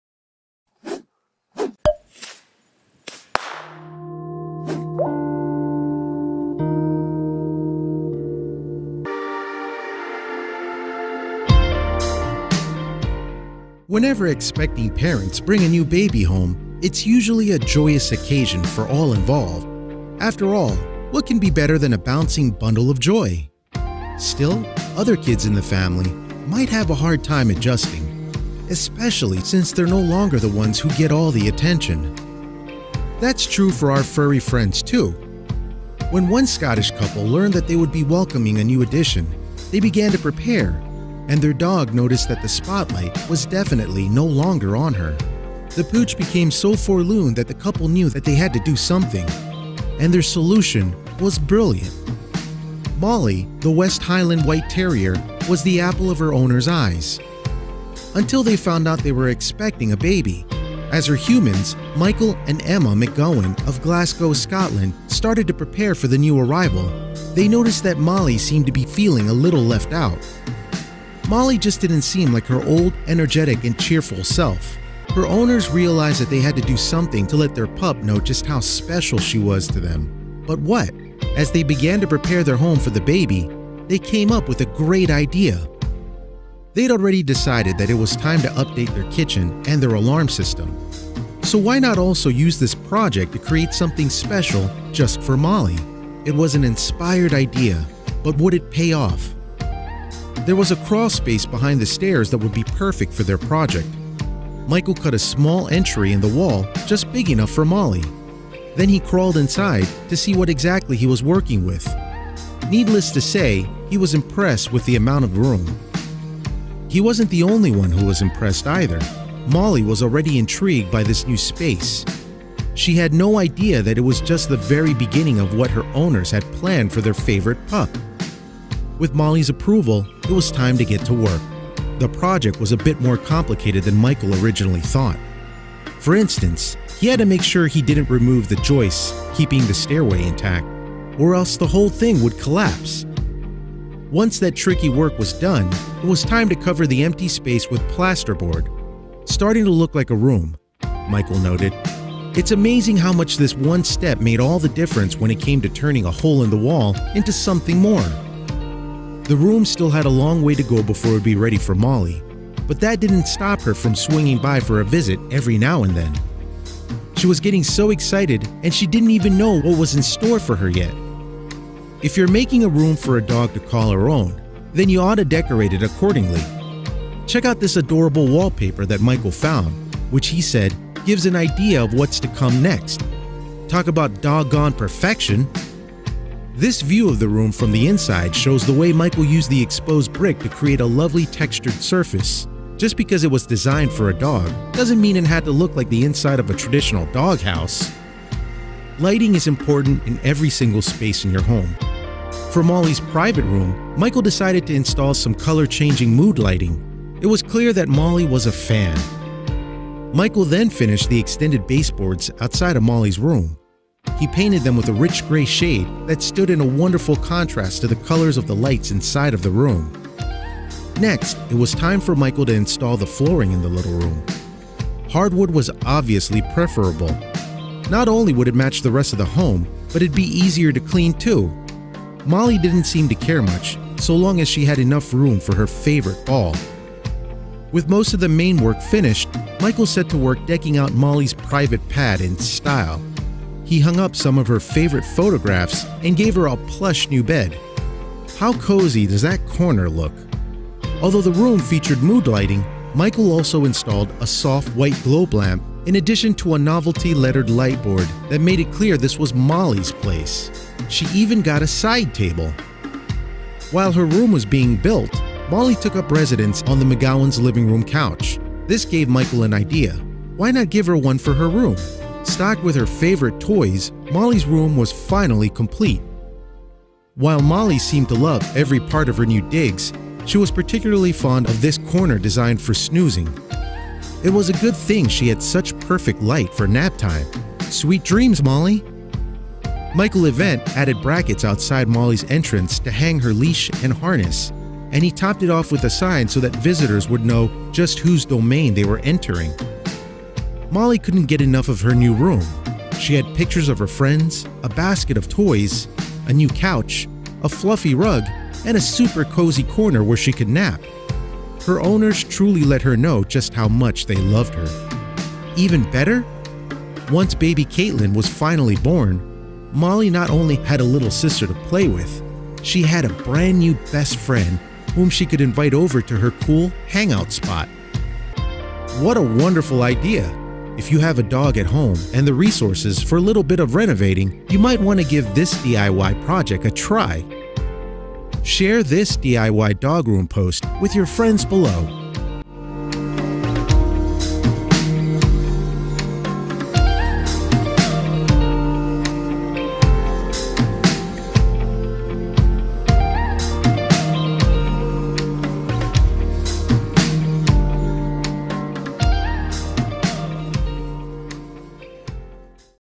Couple Expecting A Baby Is Forced To Make A Drastic Decision About Their Dog
Whenever expecting parents bring a new baby home, it’s usually a joyous occasion for all involved. After all, what can be better than a bouncing bundle of joy? Still, other kids in the family might have a hard time adjusting, especially since they’re no longer the ones who get all the attention.

That’s true for our furry friends, too! When one Scottish couple learned that they would be welcoming a new addition, they began to prepare—and their dog noticed that the spotlight was definitely no longer on her. The pooch became so forlorn that the couple knew they had to do something… and their solution was brilliant.

Molly the West Highland White Terrier was the apple of her owners’ eyes… until they found out they were expecting a baby! As her humans, Michael and Emma McGowan of Glasgow, Scotland, started to prepare for the new arrival, they noticed that Molly seemed to be feeling a little left out.